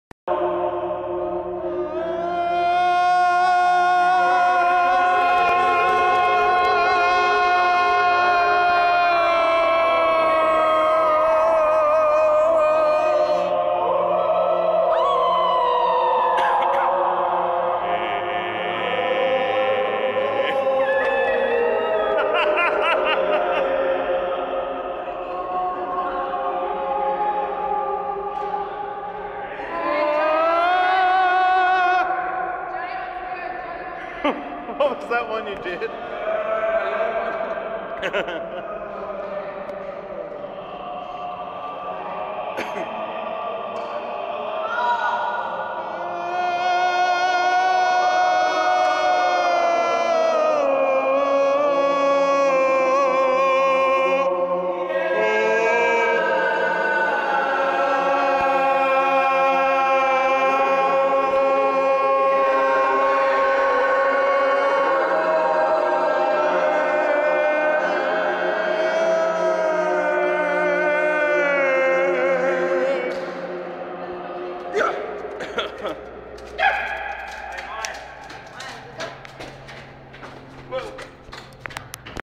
the austin, tx power plant chamber choir
apologies for my ruining most of the audio with out of tune attempts at notes and laughter.